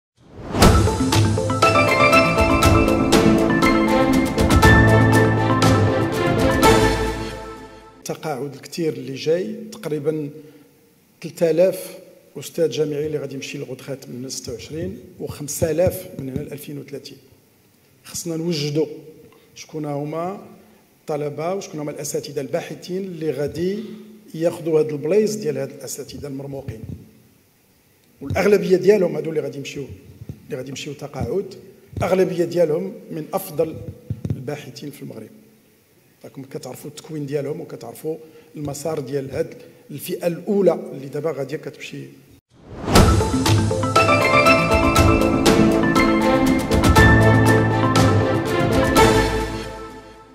تعرف على عدد الأساتذة الجامعيين الذين سيحالون على التقاعد في 2026على لسان الوزير ميراوي
في هذا الفيديو ستتعرف على عدد الأساتذة الجامعيين الذين سيحالون على التقاعد في عام 2026 و2030، على لسان وزير التعليم العالي خلال تدخله في الجلسة التي عقدها  مجلس المستشارين  اليوم التلاثاء18 أكتوبر 2022